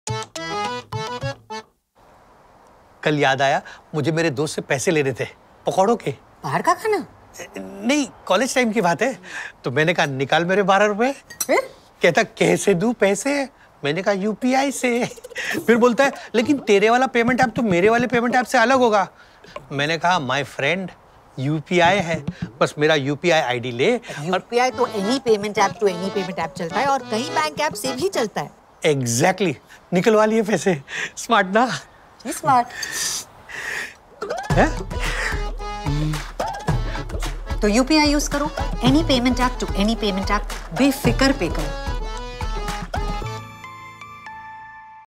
Any payment app to any payment app | UPI Chalega | Mrs. Rao & Sharma Family
Fulcro brings back Mrs. Rao & creates the Sharma family to raise awareness around UPI Safety Shield.